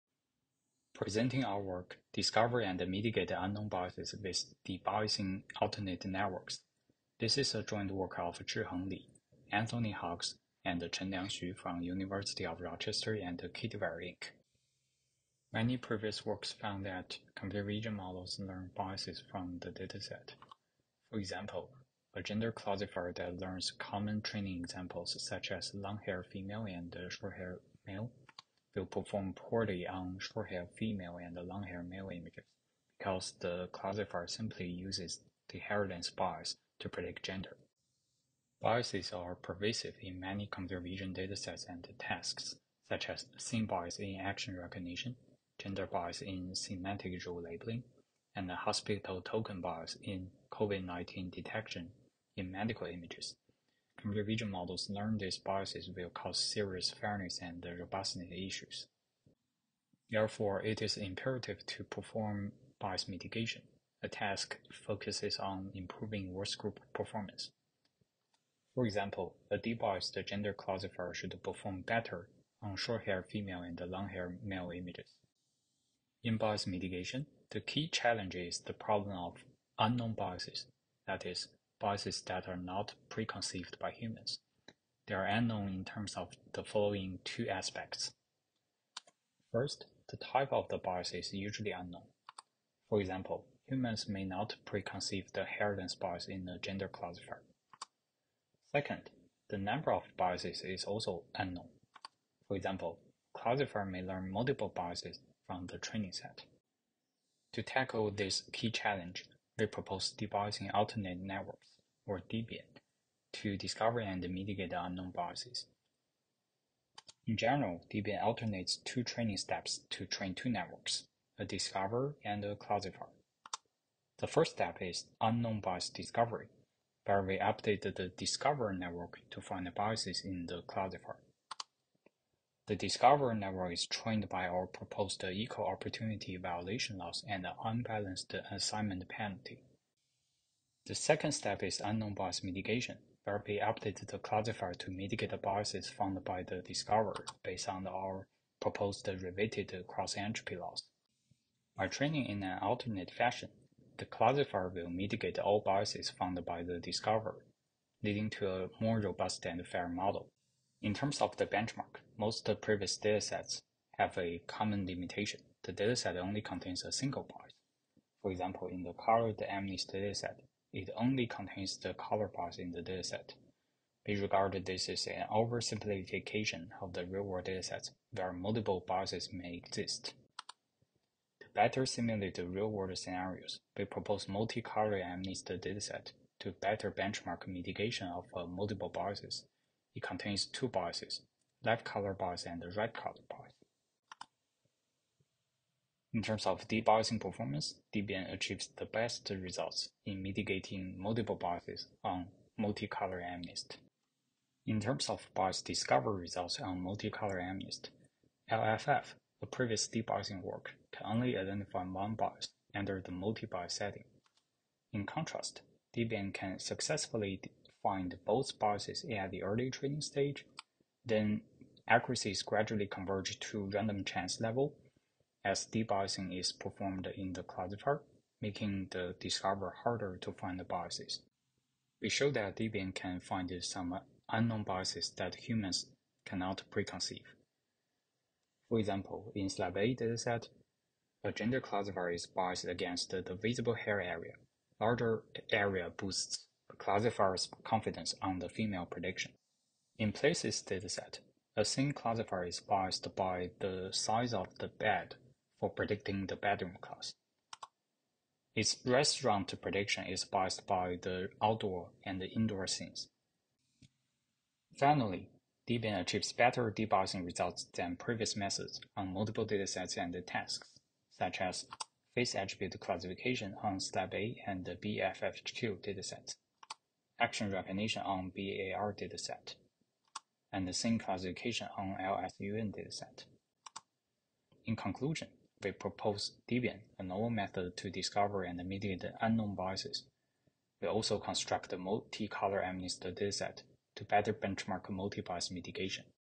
Discover and Mitigate Unknown Biases with Debiasing Alternate Networks (ECCV 2022)
Authors: Zhiheng Li, Anthony Hoogs, Chenliang Xu
University of Rochester, Kitware Inc.